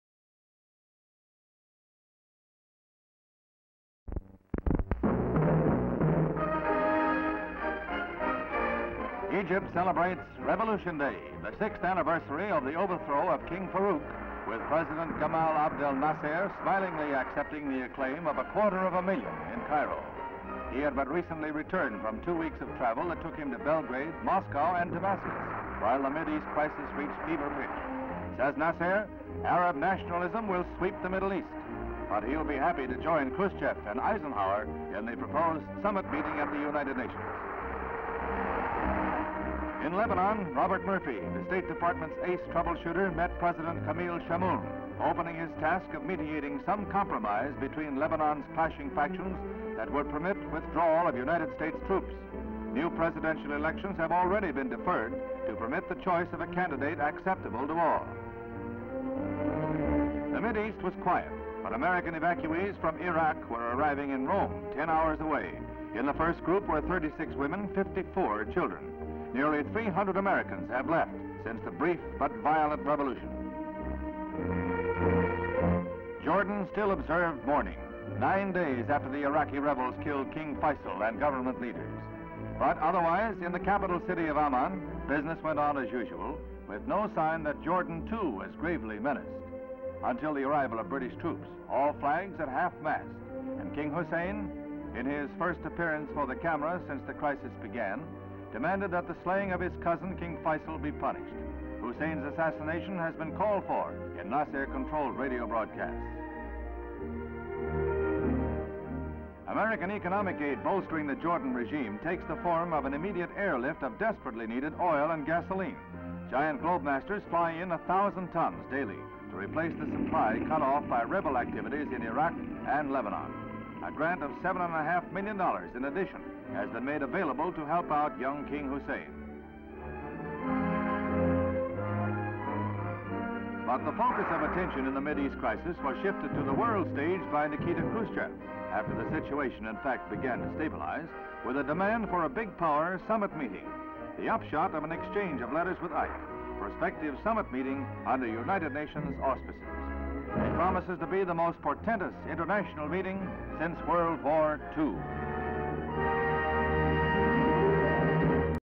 Egyptian President Nasser in Egypt, Robert Murphy in Lebanon and Soviet Premier K...HD Stock Footage
Egyptian President Nasser in Egypt, Robert Murphy in Lebanon and Soviet Premier Khrushchev at the UN summit in New York, US.

Crisis in the Middle East. Egyptian President Gamal Abdel Nasser walking with several officials. President Nasser waving to a crowd. The crowd cheering the President on the 6th anniversary of the overthrow of King Farouk I. The President speaking into several microphones from a podium. A large crowd gathered. US negotiator Robert Murphy meeting Lebanese President Camille Chamoun in Lebanon. Robert Murphy and President Chamoun with an official. The men are talking. American evacuees from Iraq arriving at an airport in Rome, Italy. Men, women and children coming down the steps from an aircraft. The evacuees at the airport. A street in Jordan. Vehicles moving on a road and people on a street. Buildings on the street. Flags flying at half mast on several buildings in the event of King Faisal II and other leaders being killed by Iraqi rebels. King Hussein of Jordan addressing a press conference. Several officials at the conference. A United States Air Force aircraft at an airfield in Jordan. Men unload gas cans from the aircraft. Men standing in a row pass the cans. Soviet Premier Nikita Khrushchev during summit talks in the United Nations. Aerial view of the United Nations building in New York, United States. Location: Middle East. Date: July 24, 1958.

57,000+ broadcast-quality historic clips for immediate download.